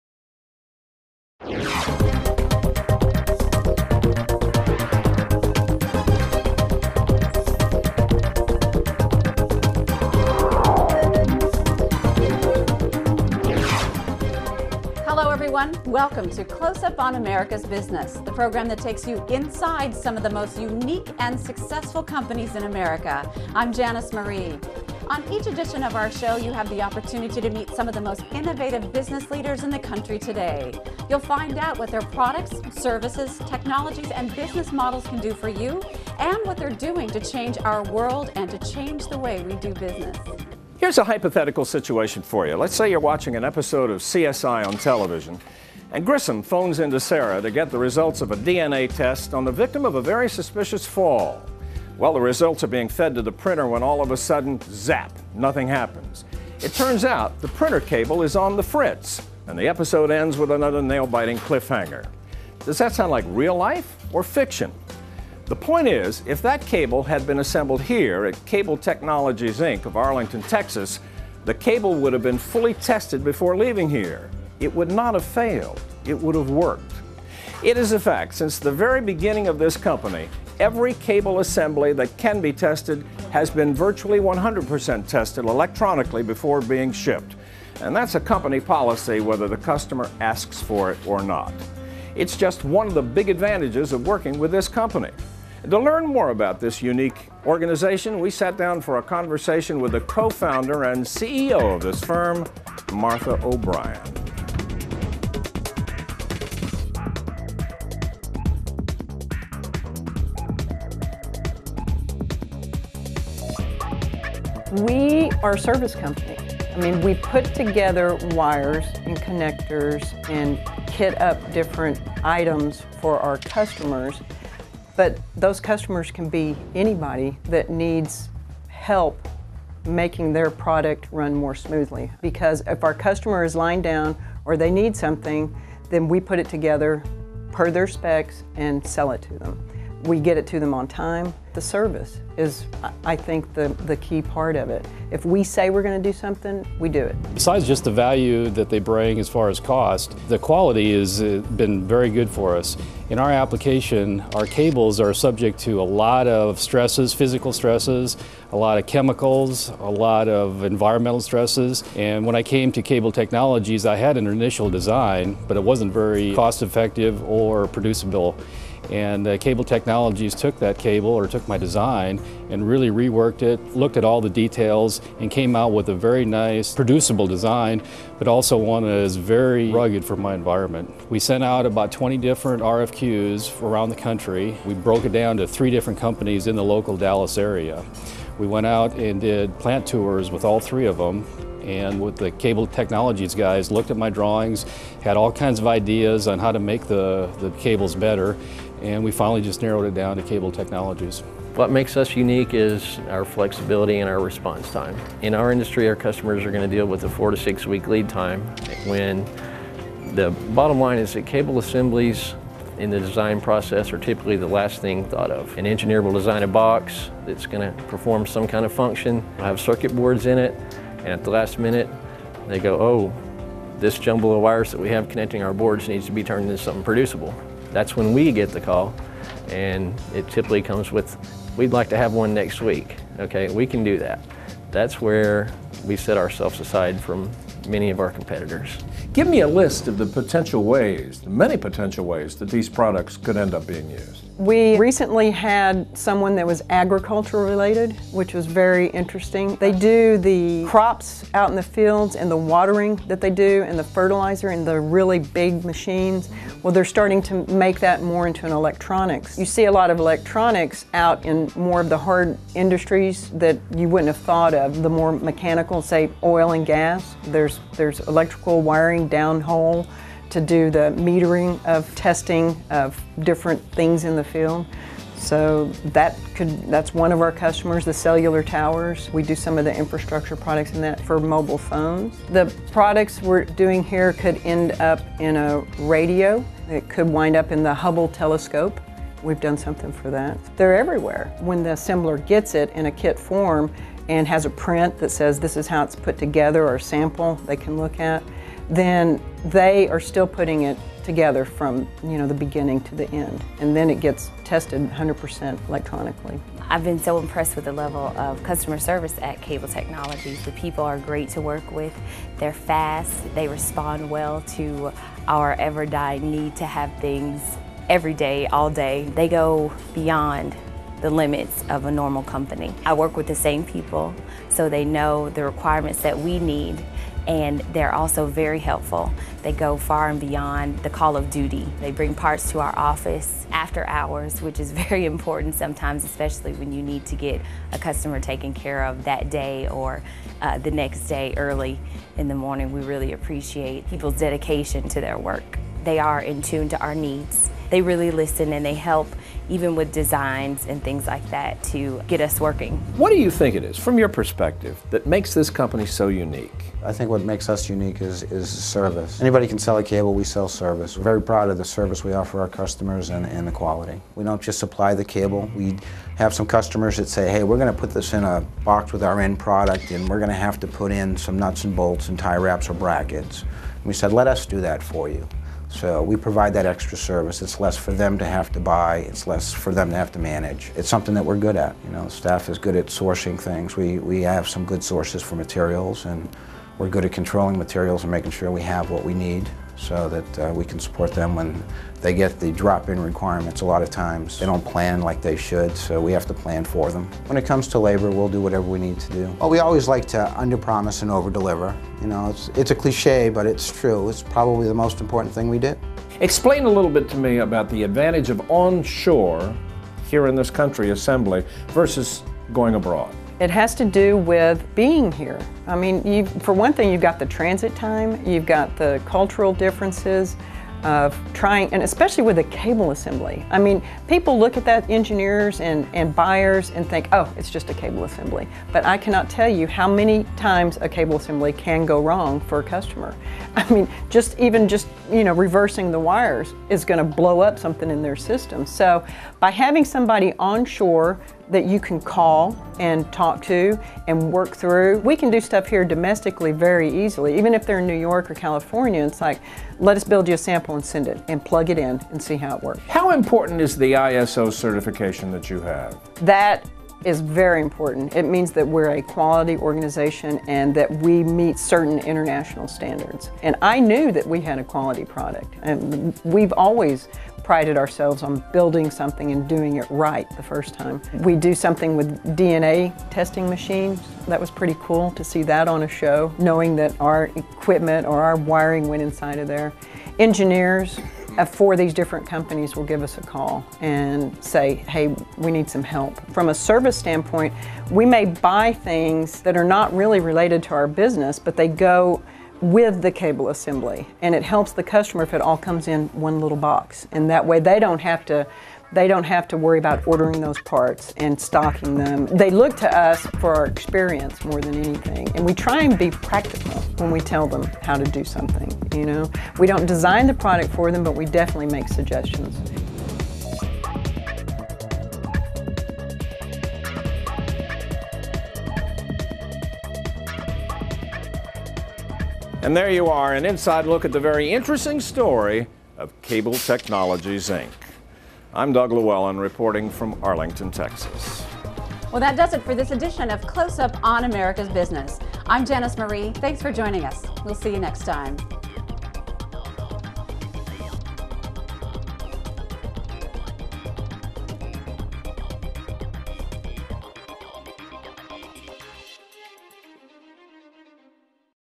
CloseUp on Cable Technologies
Close Up TV News takes you inside of Cable Technologies, Inc.

A Texas based manufacturer of custom wire harnesses and cable assemblies still growing in today's global market.